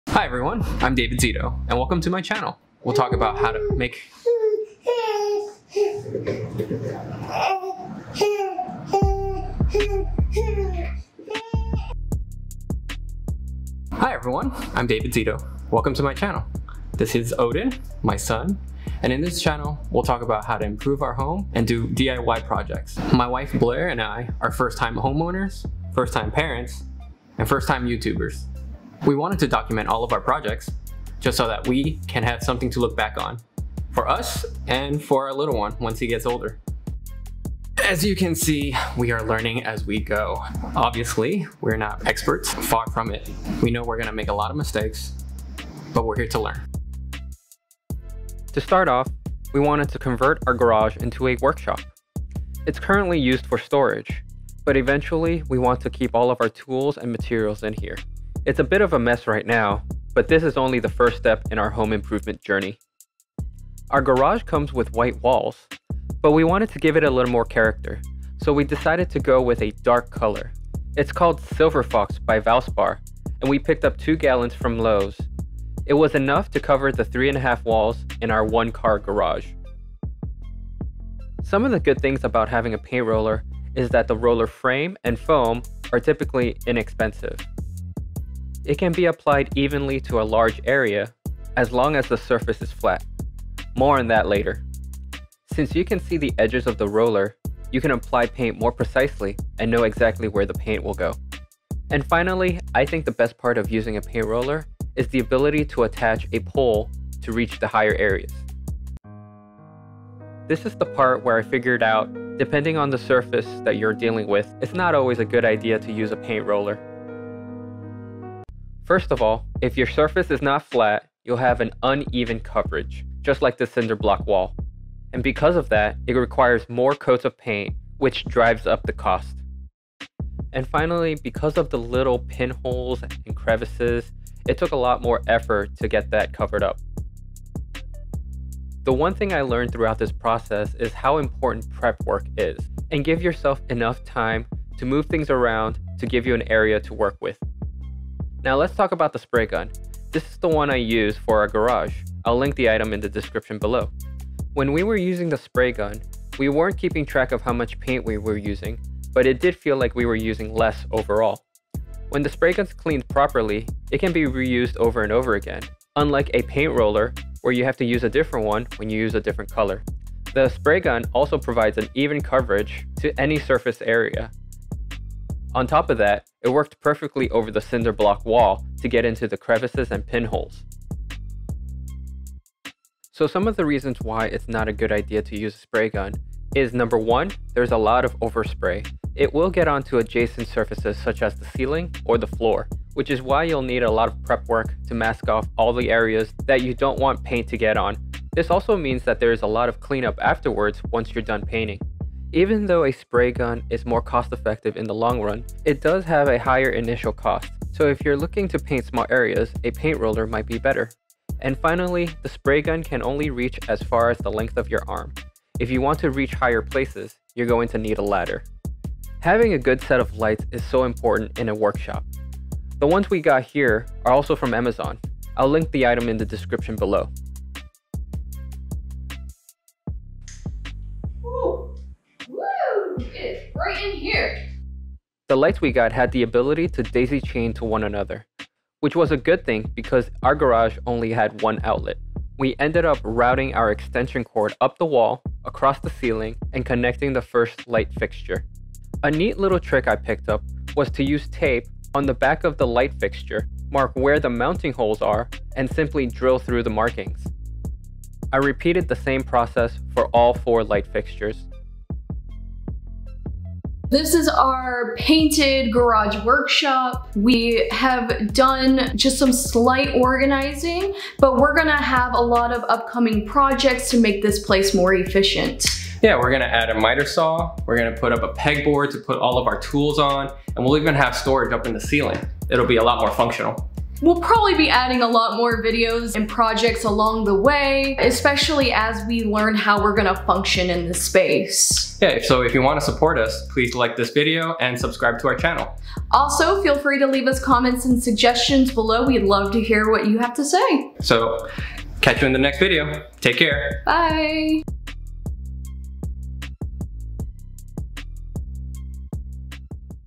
Painting With Spray Gun vs Paint Roller | New Garage Lights | Garage Update #1
Welcome to our first video on this channel!
This is where we clean up, organize, paint, and add lights to our garage to prepare it for future home improvement projects!

We tried out two methods of painting our garage: Using a traditional paint roller and a paint spray gun we bought online. They have their pros and cons. Let us know which do you prefer in the comments!

Please click the like button if you learned something new.

We are on all social media platforms @SzetoCo or follow this link for the complete list
Items from the video: (Unaffiliated links)
1. Spray Gun - "Dicfeos Paint Sprayer"

2. Lights - Bbounder LED Utility Shop Light